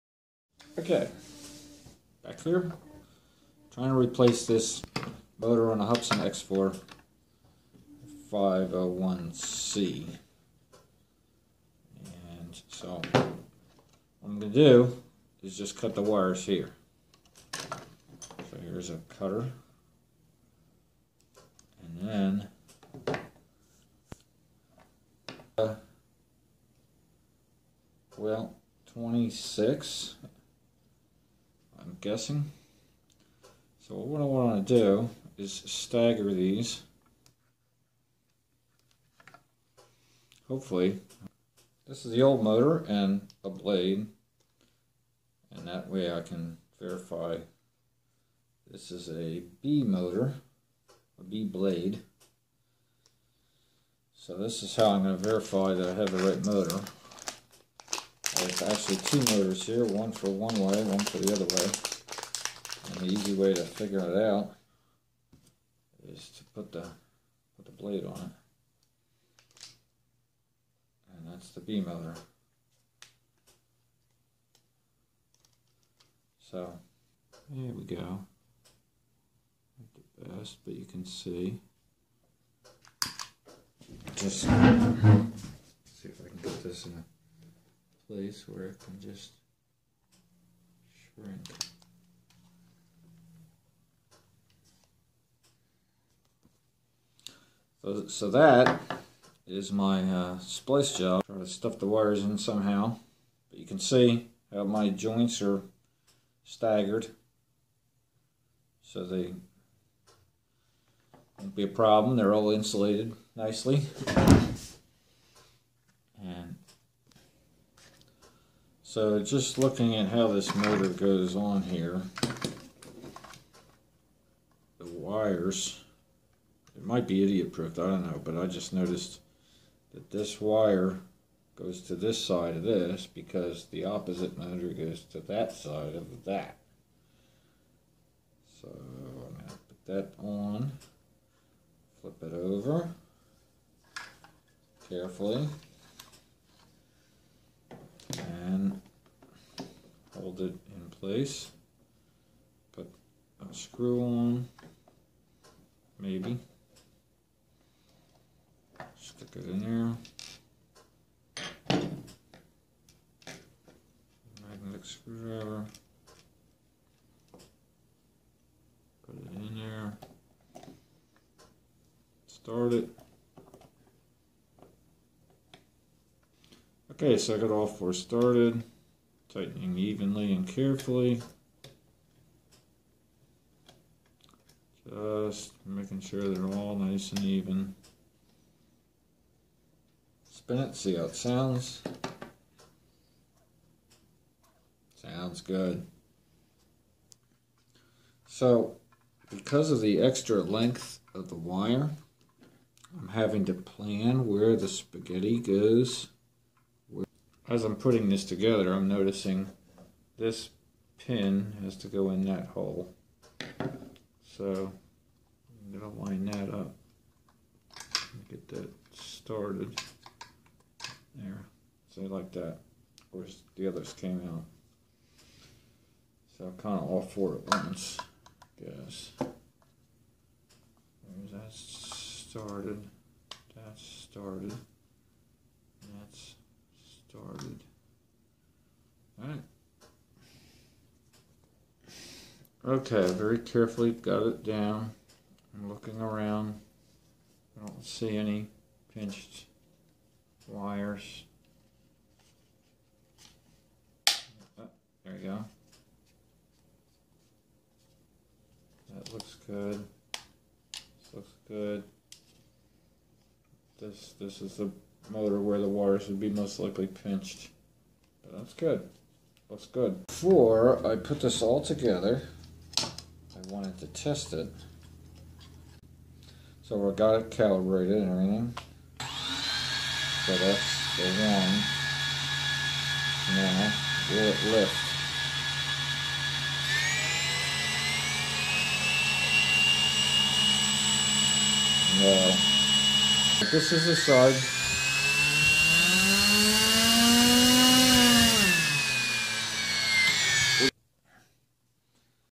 How not to replace a motor on a Hubsan H501c
This video is of installing a new motor on a Hubsan H501c drone.  It appeared to be weak because the TX or transmitter was the problem.  A second TX from a lost H502E worked fine with this H501c.  It would have been better to unsolder the wires of the old motor at the ESC board rather than cutting them and connecting to them.  To see how to get the old motor out see my video "Hubasn H501c motor removal."  Replacing the motor was ultimately unnecessary.  If you have a weak motor test before replacing.  The voltage at the motor side of the ESC between the red and black wire was 3.4 volts alternating current (AC) when the motors are armed. At Full Throttle the voltage is 5.7 volts ac on the three good Motors. But only 3.4 volts ac on the ESC red and black wire to the weak motor.  The creator of this video is not responsible for personal injury or property damage allegedly caused by this video or the suggestions in this description.